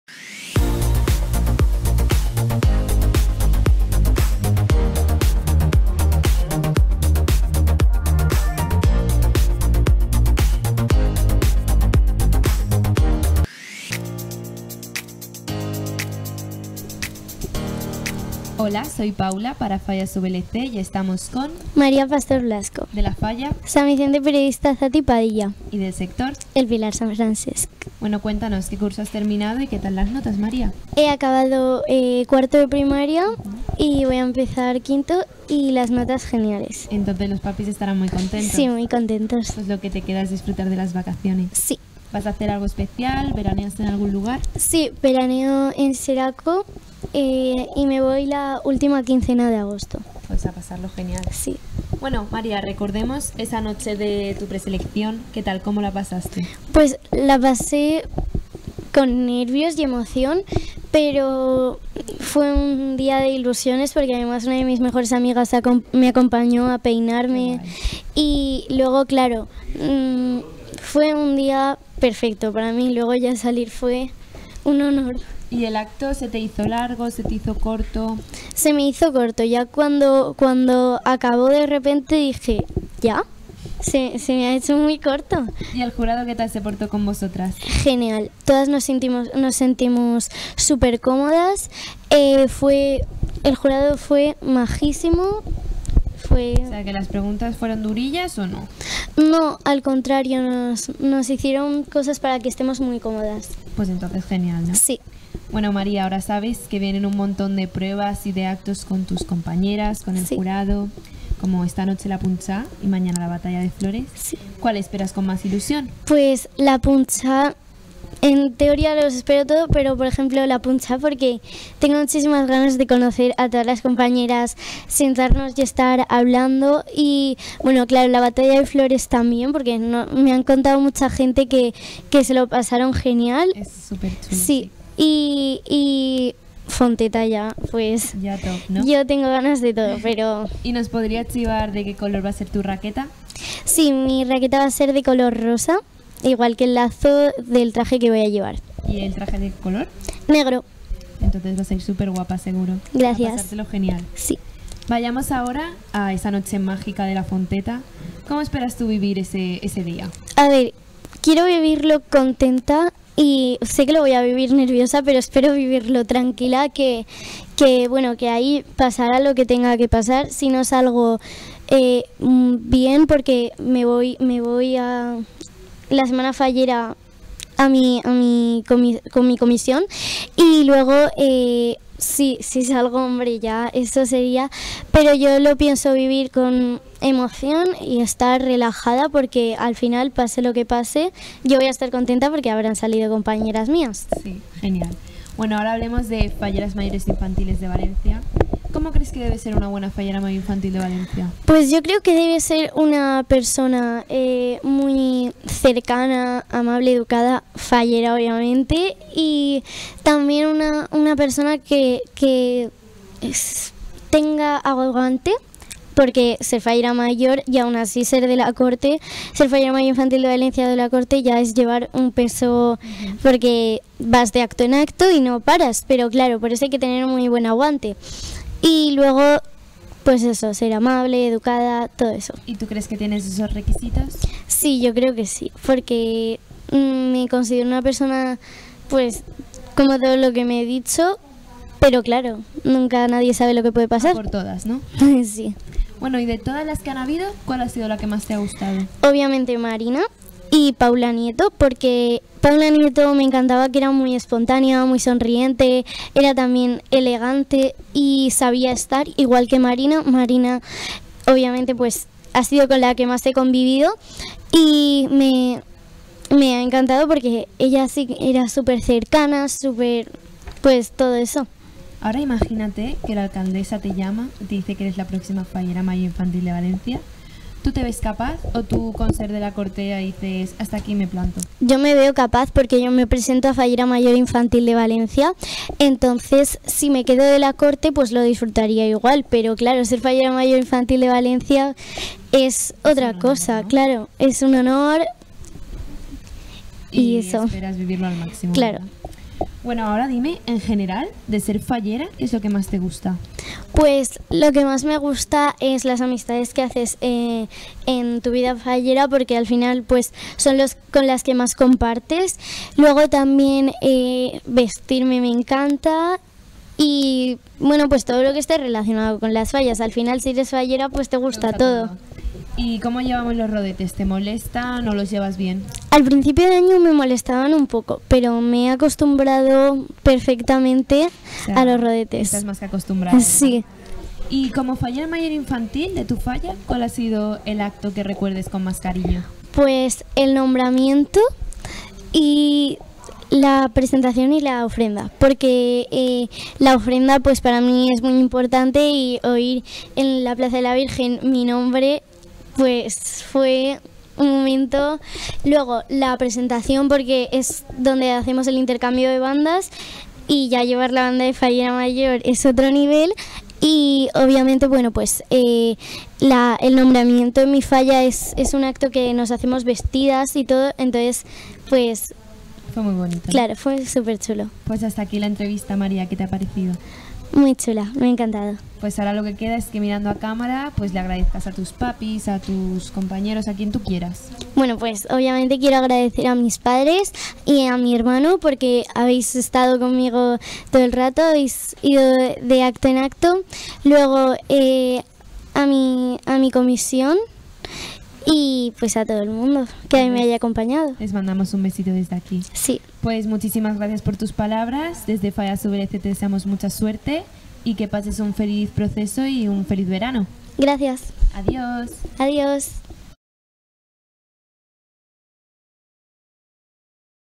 Paula Miquel – Fallas VLC – Entrevista a María Pastor Blasco, S.Vicent-Periodista, Pilar S.Francesc
Paula Miquel – Fallas VLC – Entrevista a María Pastor Blasco, Sant Vicent-Periodista, El Pilar Sant Francesc